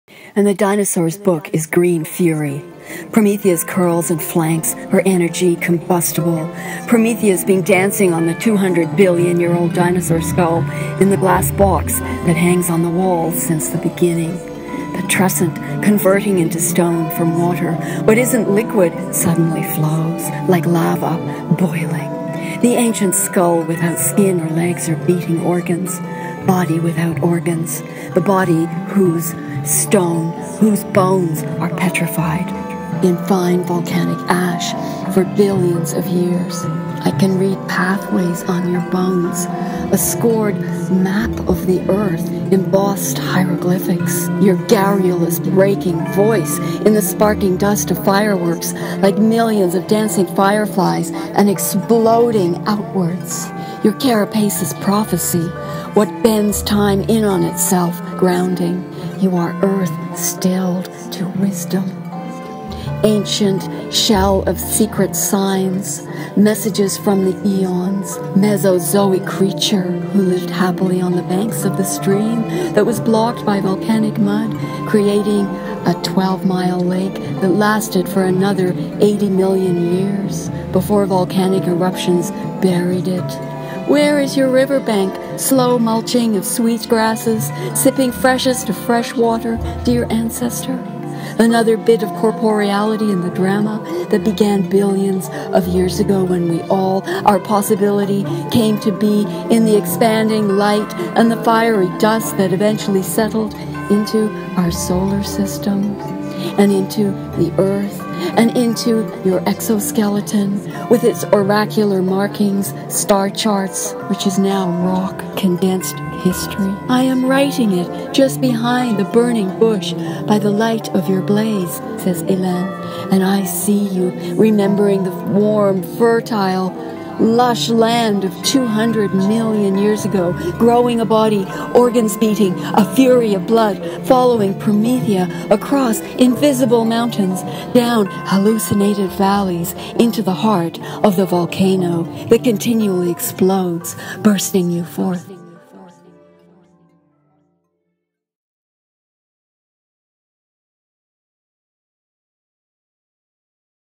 The Dinosaur Book Is Green Fury
A time-lapse art video: drawing in India inks in my beloved Moleskine Folio Sketchbook A4; pulsing green kalaidoscope in the background; text of the poem moving slowly up the screen at a diagonal; and a voiceover poem. The world is a green furor of creativity - the green fire of life.

I shot the video with a Canon HF S100 and speeded up about 800%.

Twenty min of footage became a 2.5 minute video. A longer drawing would use a huge amount of space on the hard drive, and so, except for short films, I don't recommend this technique.

I edited the footage in Final Cut Express 4.0.1. Because of the camera angle, I rotated and cropped the sketch clip, and underneath added a layer of footage with a kalaidescope filter, and also ran the text of the prosepoem over the paper at an angle, motion keyframing it, and changing the opacity from light to dark letters over the duration of the video.

I created the music in a cool program, a 'P22 Music Text Composition Generator (A free online music utility)' In this program, each letter has a sound. When you put text in, you can choose what BMP and instrument you'd like, and the program generates a midi file, with the sheet music. I layered my track in GarageBand 6.0.2 using different instruments, splicing and re-arranging.

Even the reading of the writing was speeded up, in Audacity 1.3.12, using the tempo filter.

From start to finish took about 12 hours, there were many layers, of image, text, and sound, each with filters, and I had to render a few times, which took hours, to see if what I had produced worked.

While this method for creating an art video works, my camera battery can only tape for 1½ hours, which is not long enough for most art projects.
Rubies In Crystal post, with the full text of the prose poem

This video poem was featured at Moving Poems, an "anthology of the best videopoems, filmpoems, animated poems, and other poetry videos from around the web"